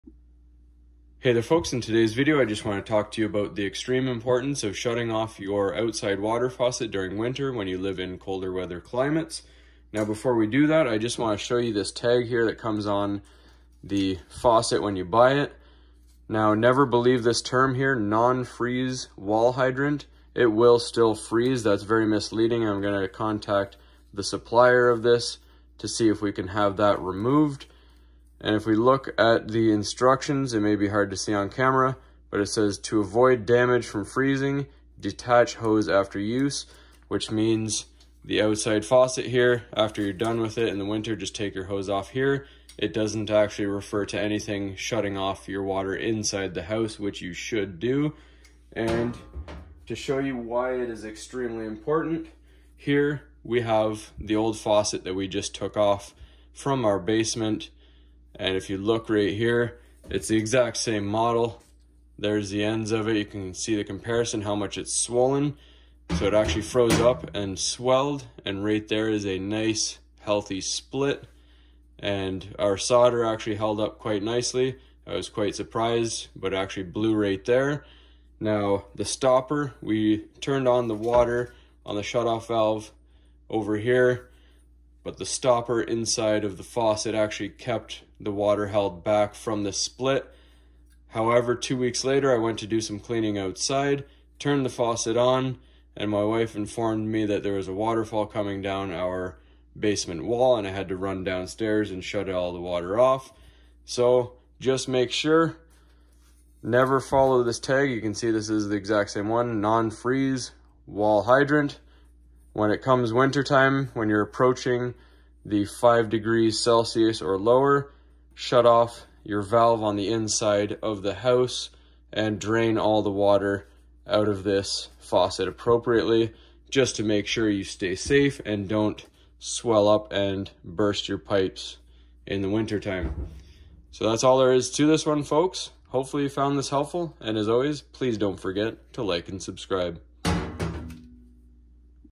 PSA: IMPORTANCE OF SHUTTING EXTERIOR FAUCETS OFF IN WINTER!
What happens if you don’t shut off your water to you exterior water faucet or hose bib? Take a look at what damage this mistake will cause